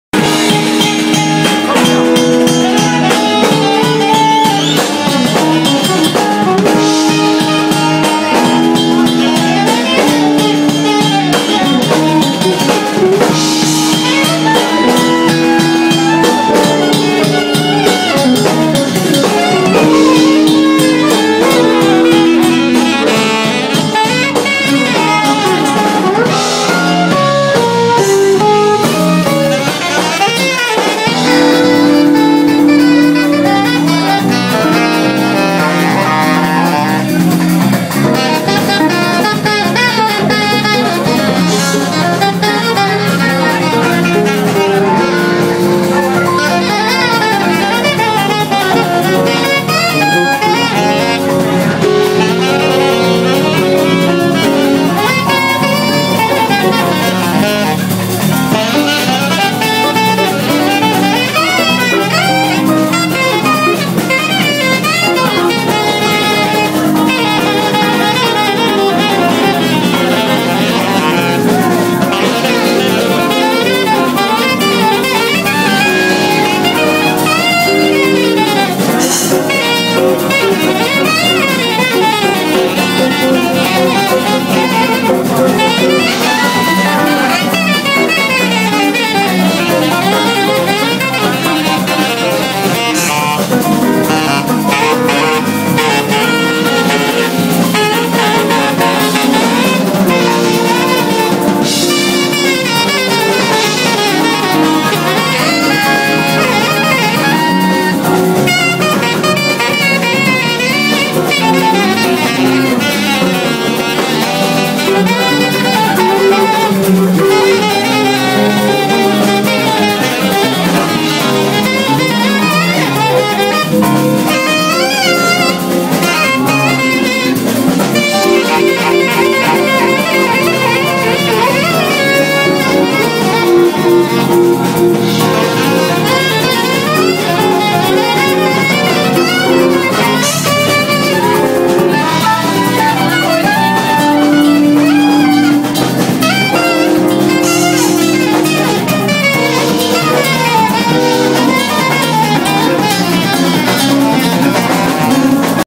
Michel Castellanos' night @ Charlie Wrights, London UK
Michel Castellanos drums
20 June 2013
Alan Short sax
Rick James bass
Aleph Aguiar guitar
Tomasz Bura keys